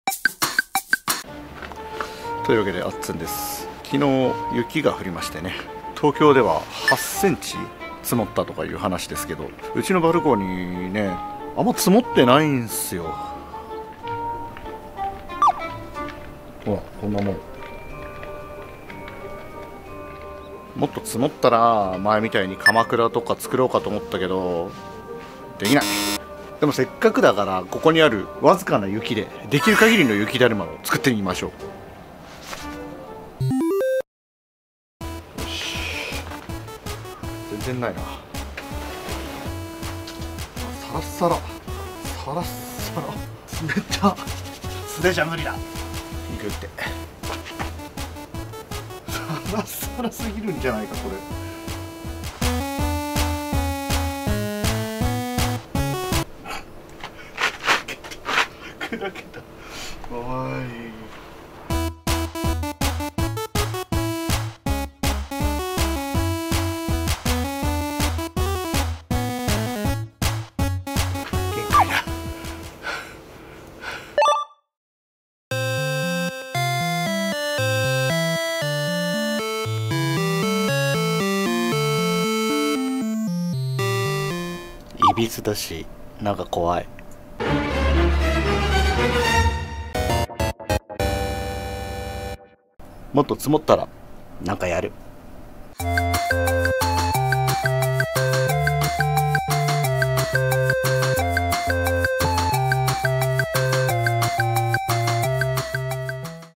精一杯の雪だるま【ふらっと】
あんまり積もらなかったけど、がんばって雪だるま作りました